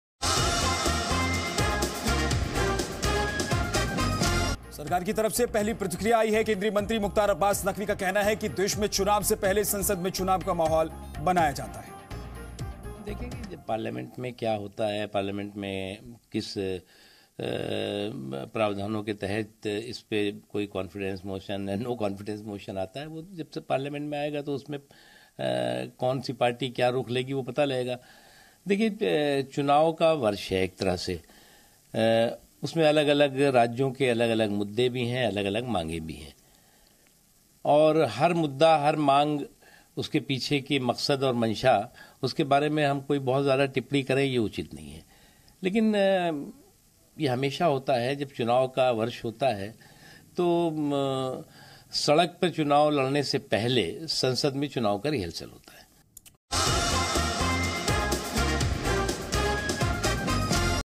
Mukhtar Abbas Naqvi reacts to TDP's retreat from NDA government
Mukhtar Abbas Naqvi reacts to TDP's retreat from NDA government. He said that before elections actually take place, an environment of such is made in the Parliament. Watch to know more.